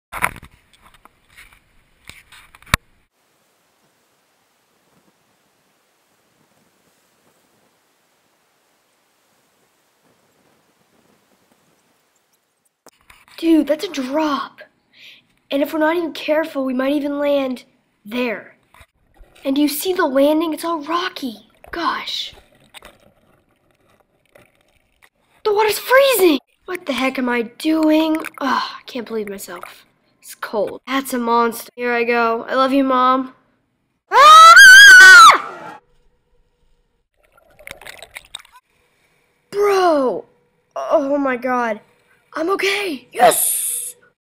cliff diving
cliff diving off 1023 feet high